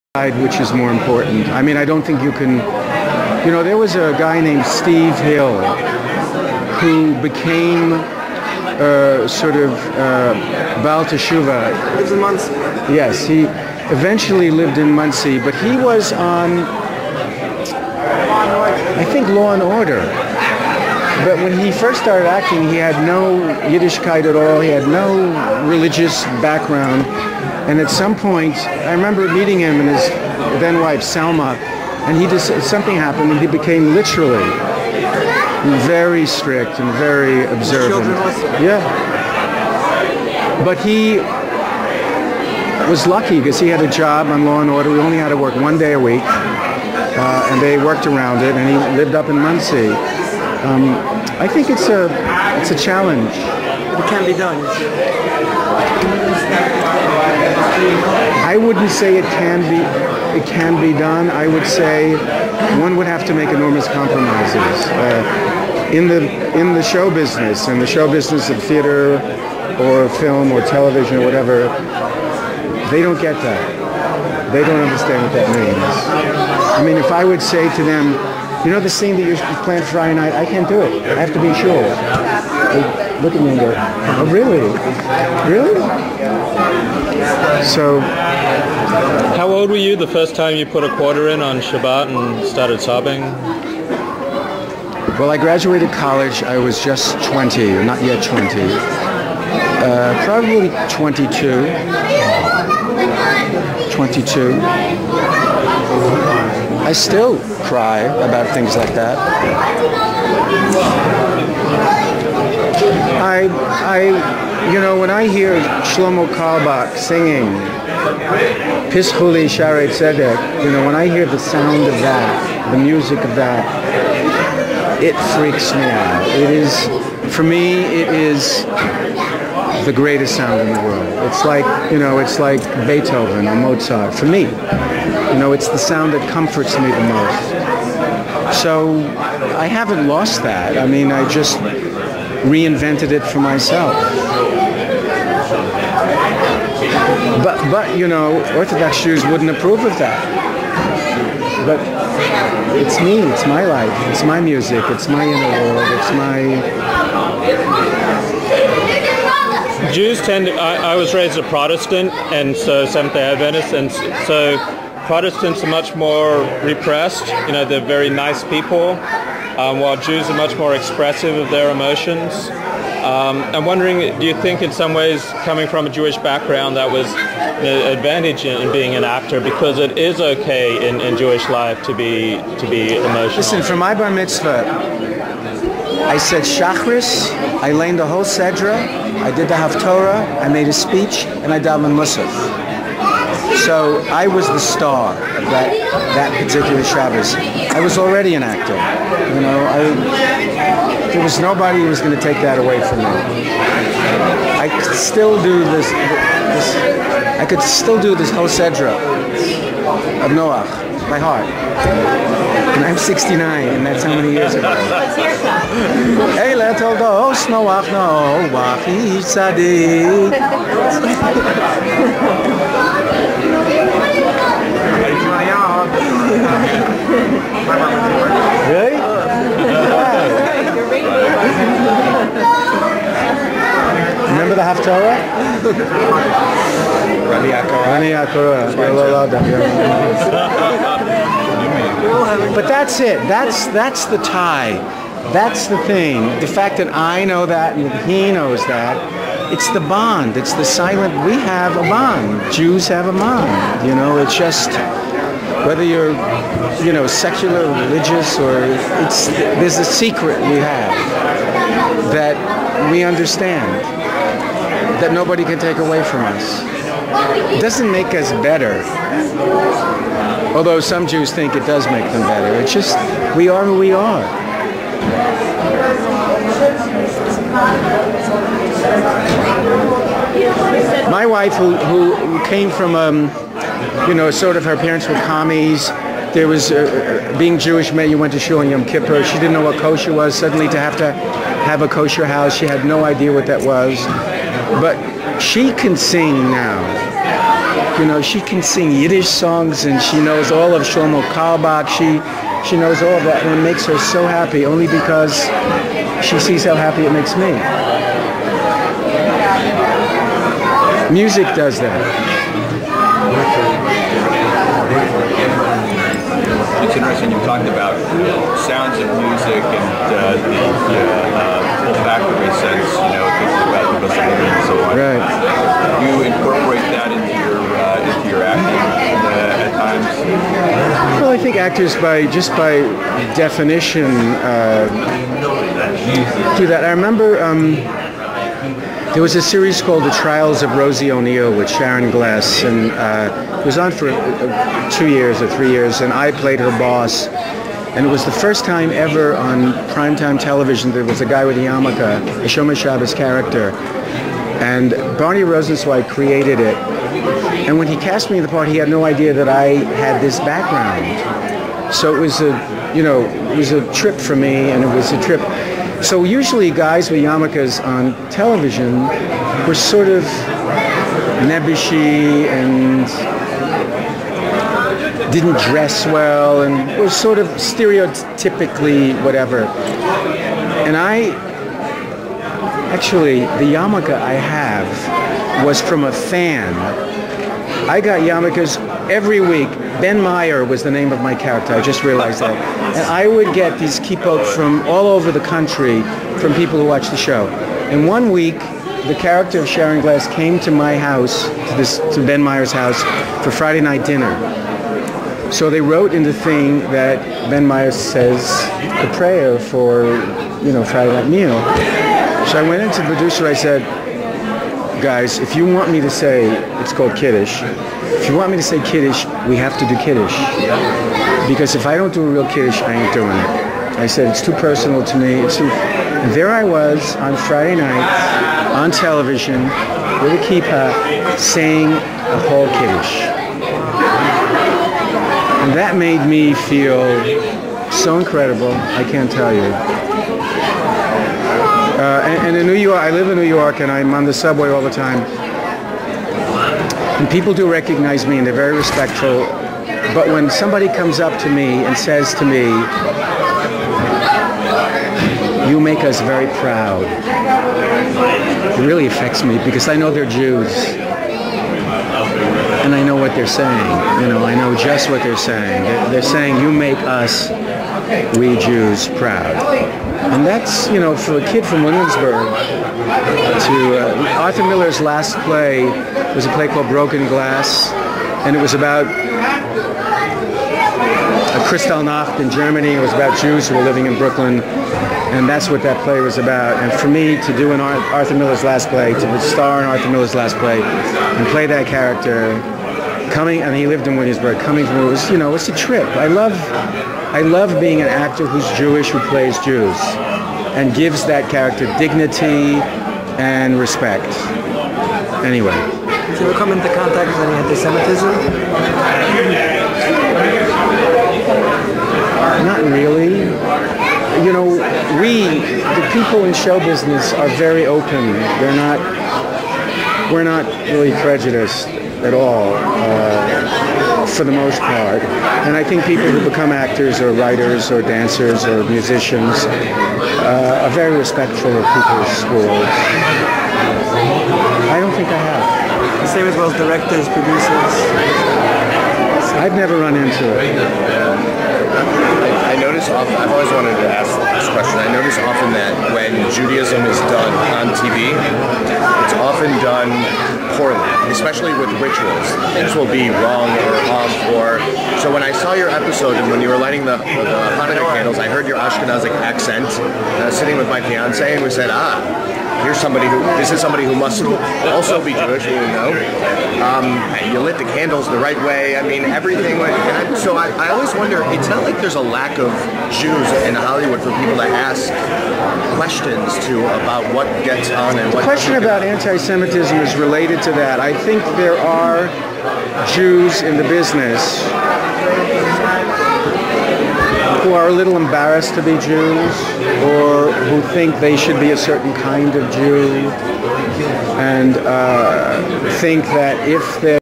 Actor Ron Rifkin Speaks About His Jewish Journeys At LimmudLA Feb. 17, 2008 II
Ron Rifkin went to yeshiva in New York. He stayed Orthodox until his twenties when he started traveling on Shabbos to work. He practiced under a Hindu monk for five years after leaving Judaism. He reflects out loud.